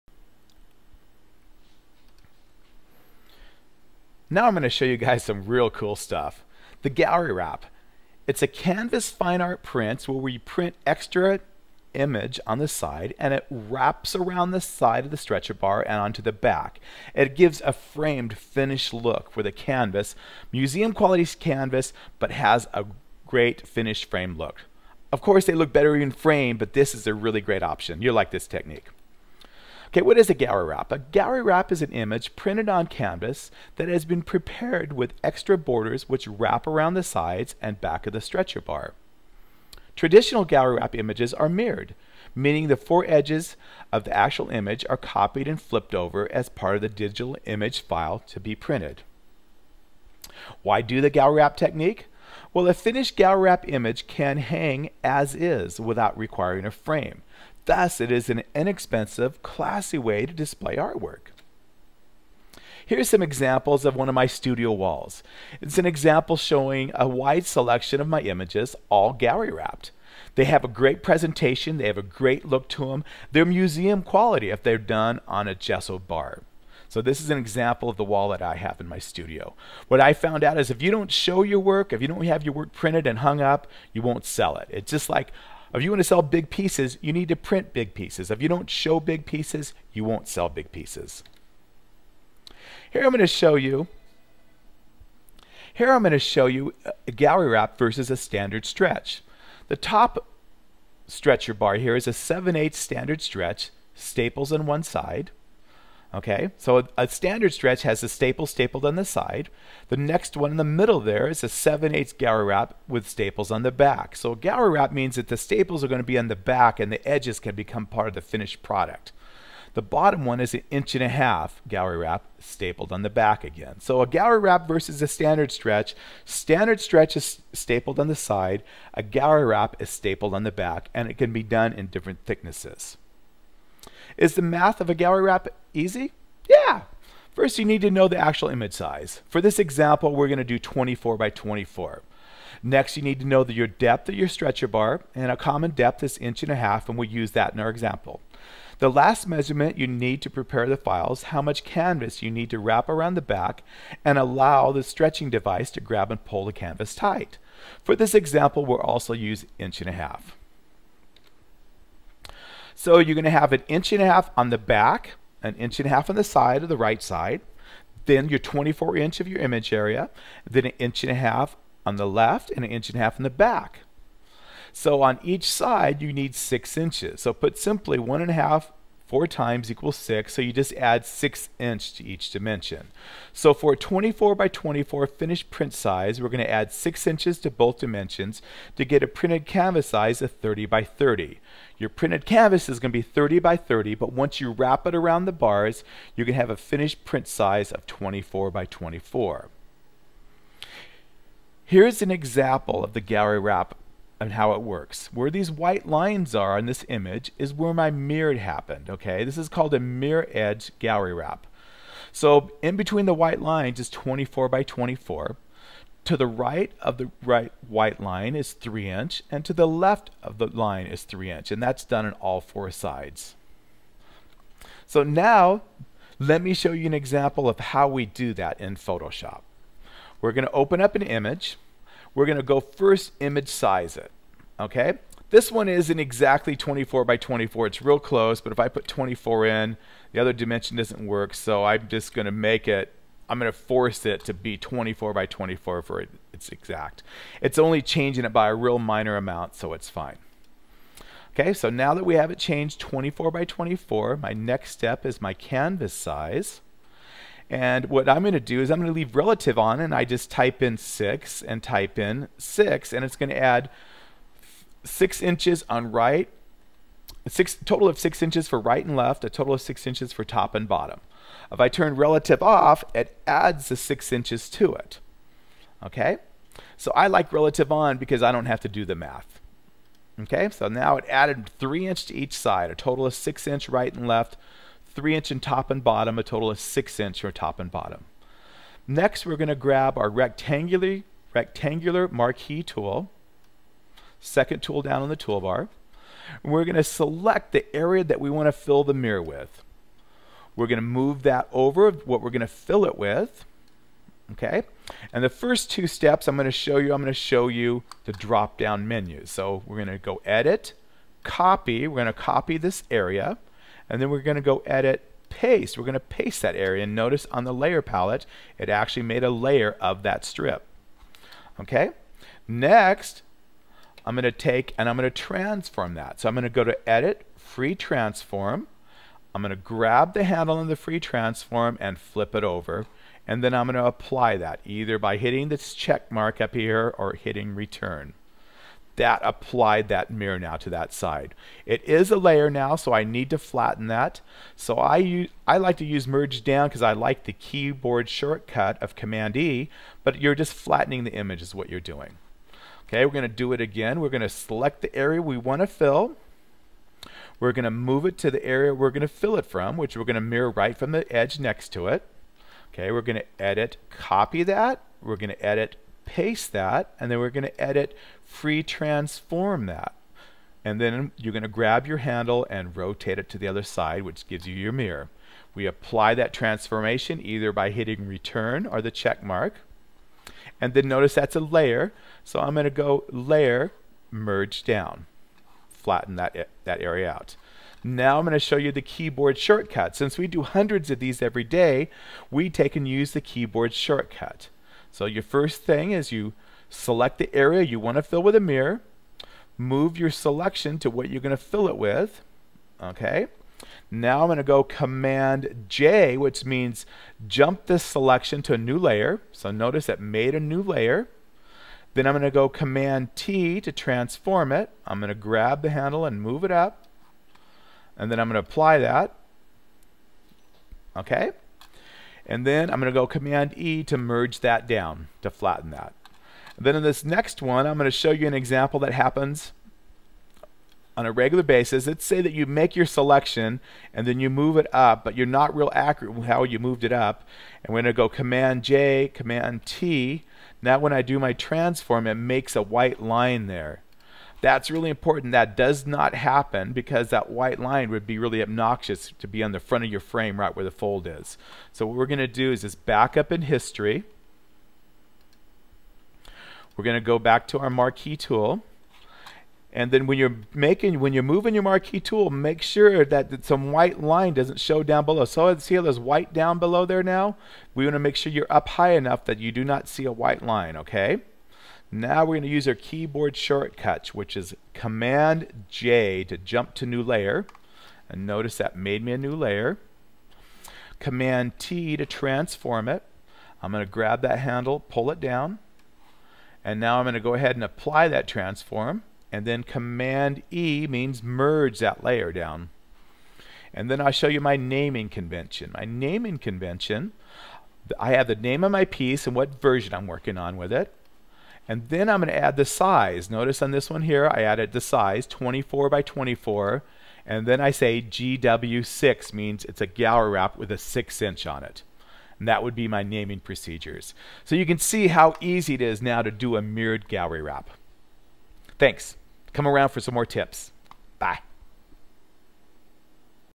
Gallery Wrap : How to do the Photoshop Technique for a canvas gallery wrap
How to do a Gallery Wrap Photoshop Technique for doing the edge mirror effect. This has step by step Photoshop technique for adding edges on a canvas for a gallery wrap.